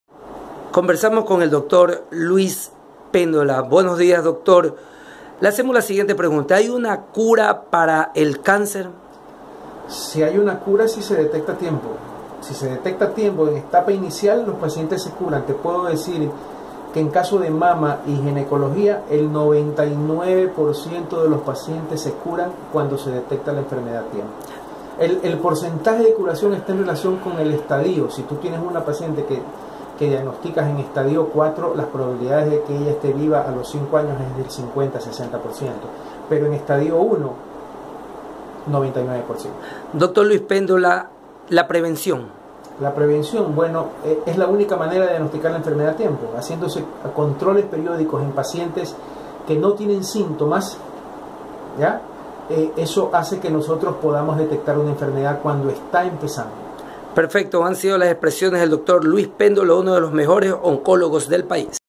El cáncer si se cura si se detecta a tiempo
Luis Pendola, Medico Oncologo de Ecuador lo expresa. Bendiciones, la prevencion es el primer paso.